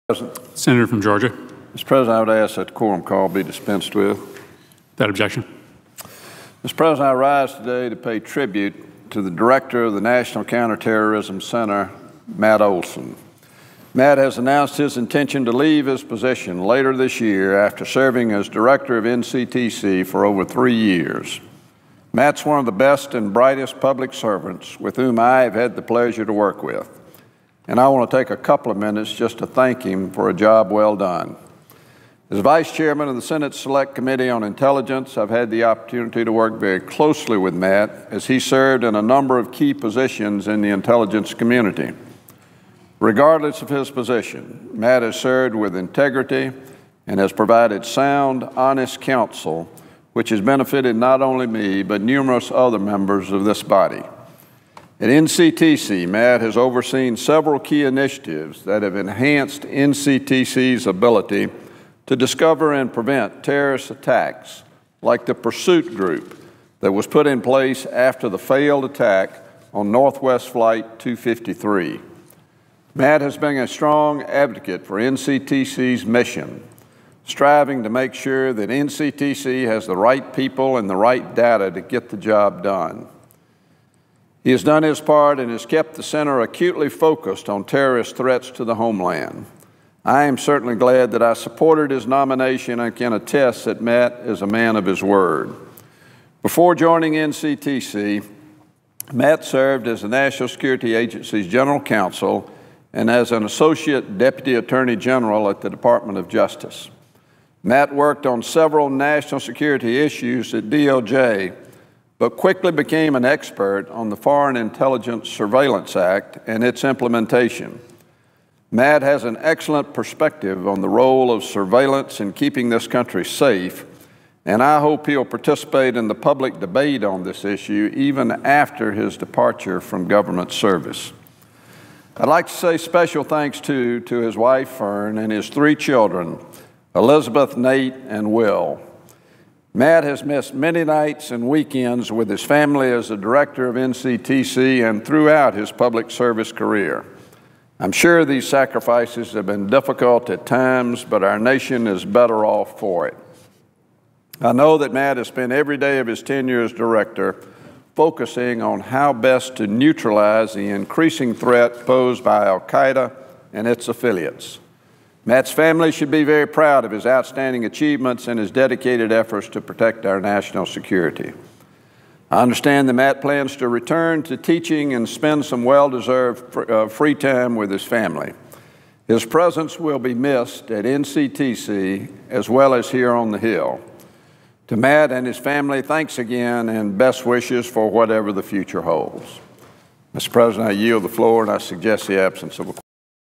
Tribute to Matt Olsen, the director of the National Counterterrorism Center
Senator Saxby Chambliss pays tribute to the director of the National Counterterrorism Center, Matt Olsen. Olsen has announced his intention to leave his position later this year after serving as director for over three years.